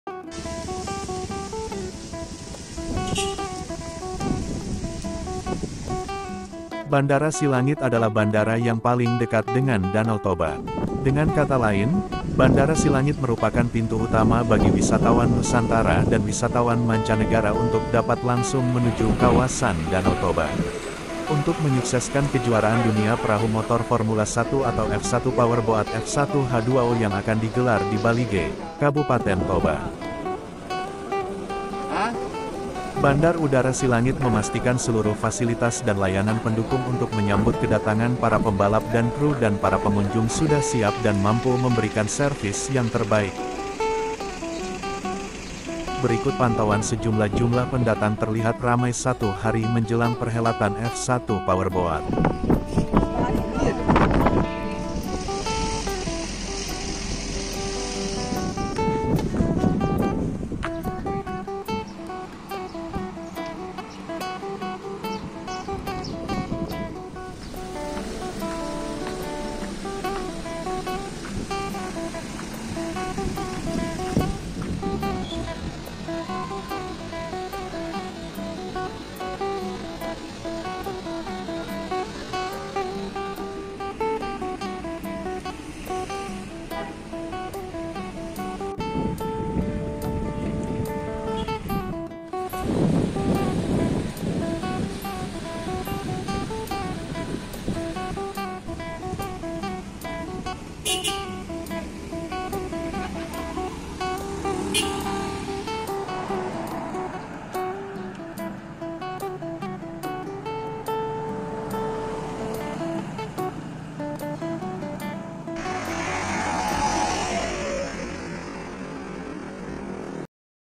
BANDARA SILANGIT DIPADATI PENGUNJUNG F1 POWERBOAT || DANAU TOBA
Pengunjung F1 Powerboat 2023 memadati area Bandar Udara Internasional Silangit, Siborongborong, Tapanuli Utara, Sumatra Utara